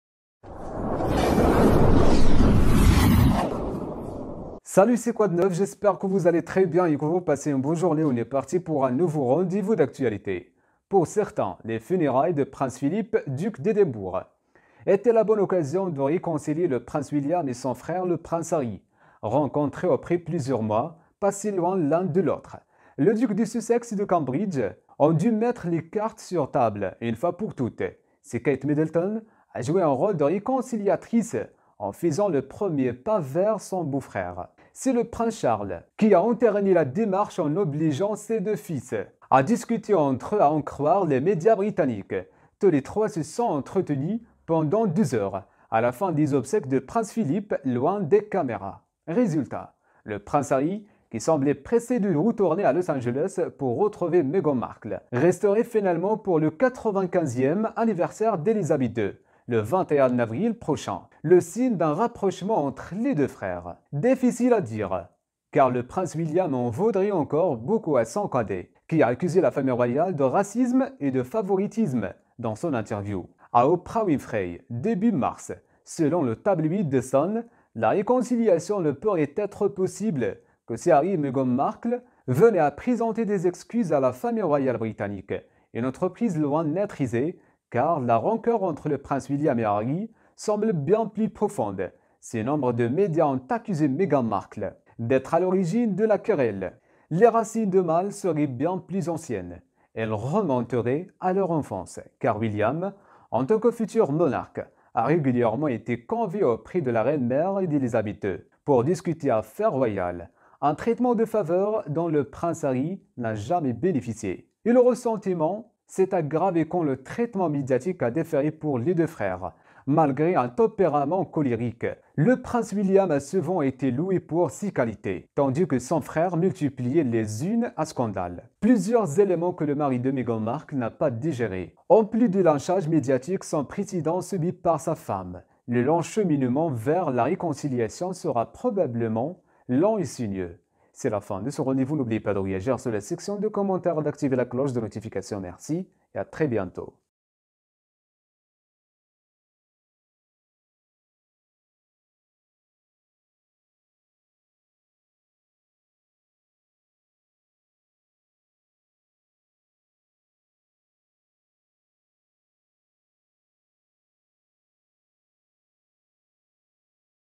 William et Harry : l'heure de la réconciliation a-t-elle sonné entre les deux princes?
Pour certains, les funérailles du prince Philip, duc d'Édimbourg, étaient la bonne occasion de réconcilier le prince William et son frère le prince Harry. Rencontrés après plusieurs mois passé loin l'un de l'autre, le duc de sussex et de cambridge ont dû mettre les cartes sur table une fois pour toutes.

Plusieurs éléments que le mari de Meghan Markle n'a pas digérés, en plus du lynchage médiatique sans précédent subis par sa femme. Le long cheminement vers la réconciliation sera probablement long et sinueux.